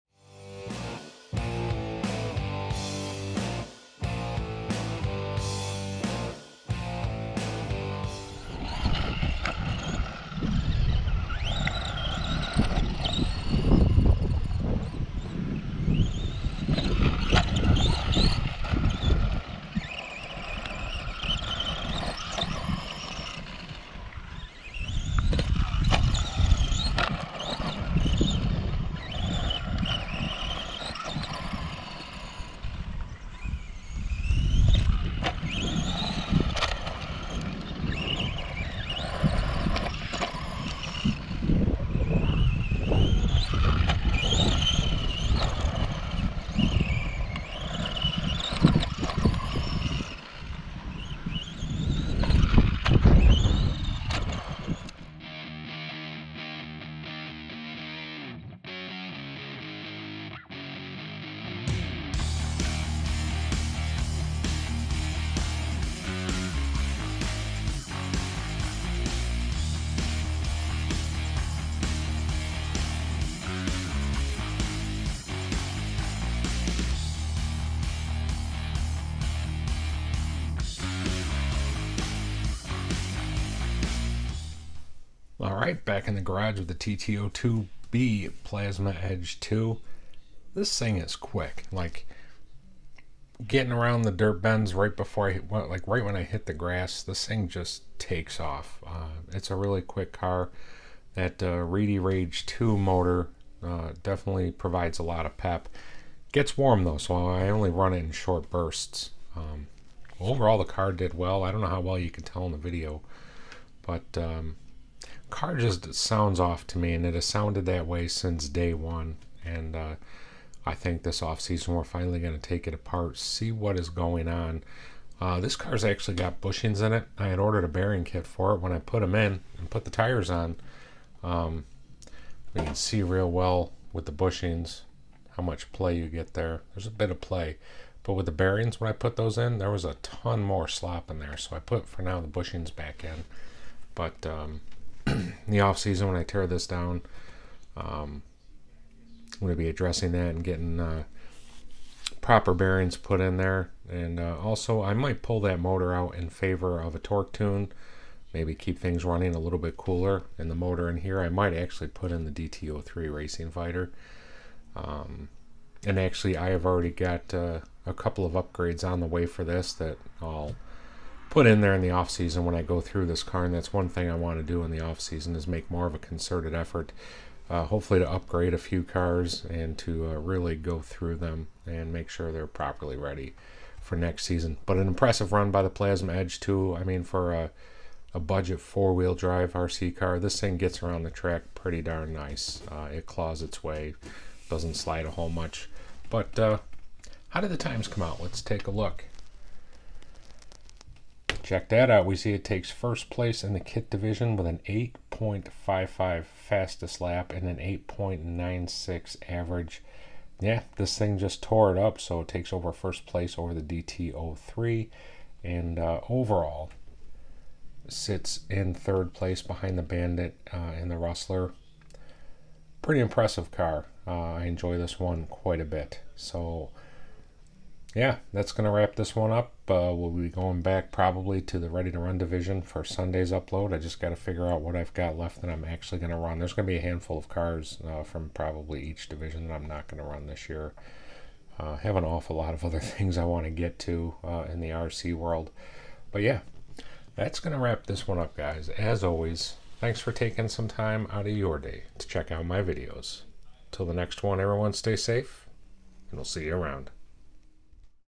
2024 time trials summer series- Tamiya TT-02B Plasma Edge II
Last years 3rd place finisher in the kit division, the Plasma Edge II, takes to the track! Can it improve upon last years finish?